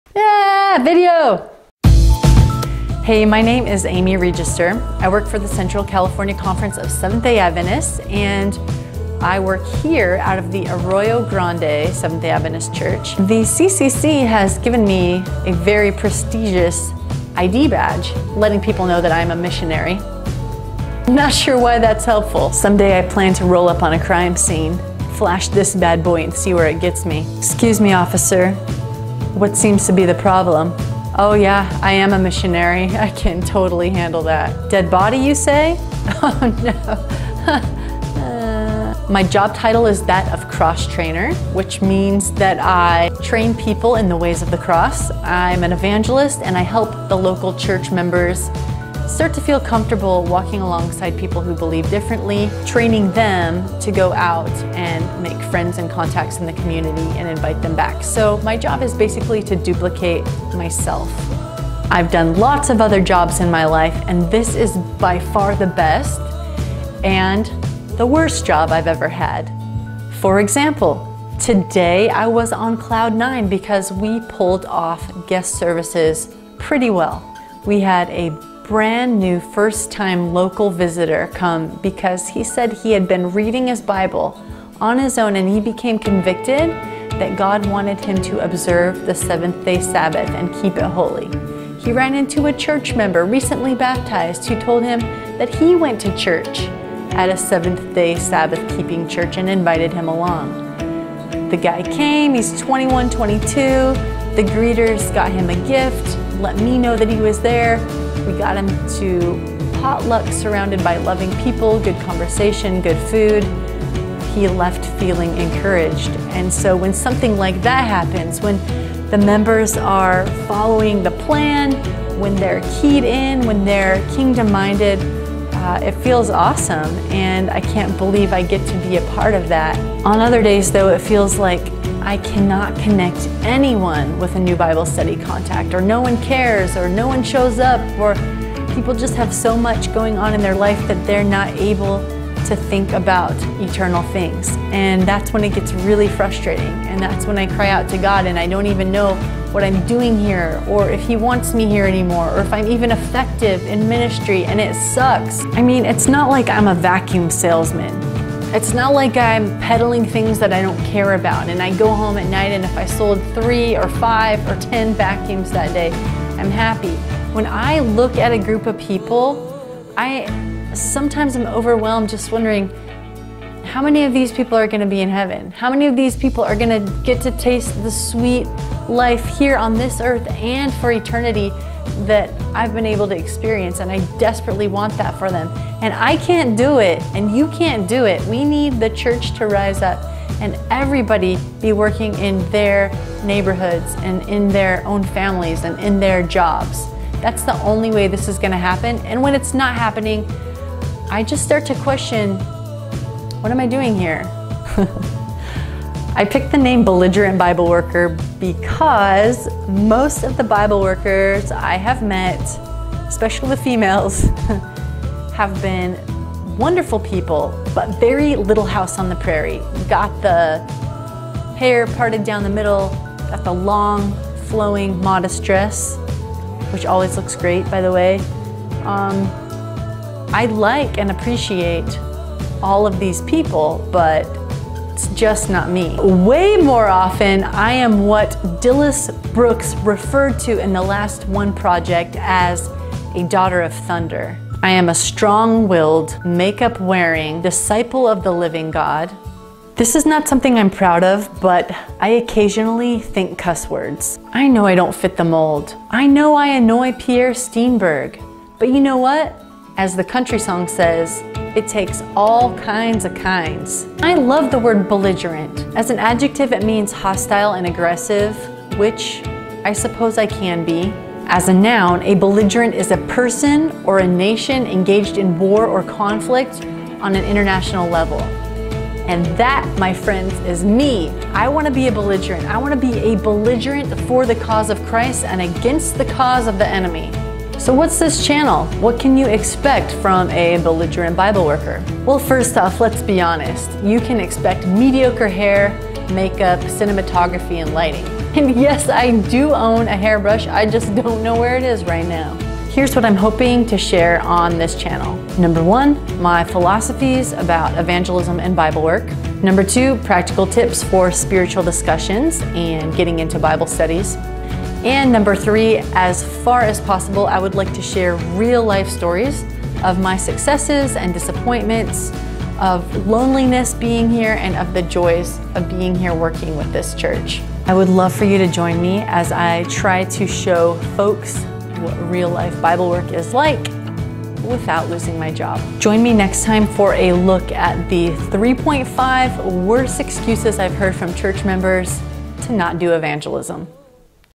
What is Belligerent Bible Worker?
I wanna keep a little vlog to tell some of the stories, victories and frustrations about Bible worker life in my area. This is the introductory video.